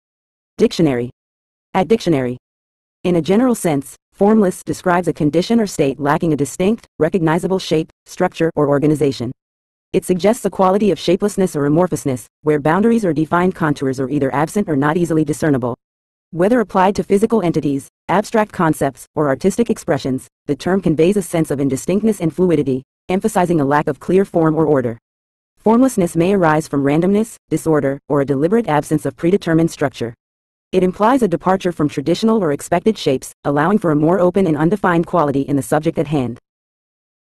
Formless Meaning In English
In a general sense, "formless" describes a condition or state lacking a distinct, recognizable shape, structure, or organization. It suggests a quality of shapelessness or amorphousness, where boundaries or defined contours are either absent or not easily discernible. Whether applied to physical entities, abstract concepts, or artistic expressions, the term conveys a sense of indistinctness and fluidity, emphasizing a lack of clear form or order. Formlessness may arise from randomness, disorder, or a deliberate absence of predetermined structure. It implies a departure from traditional or expected shapes, allowing for a more open and undefined quality in the subject at hand.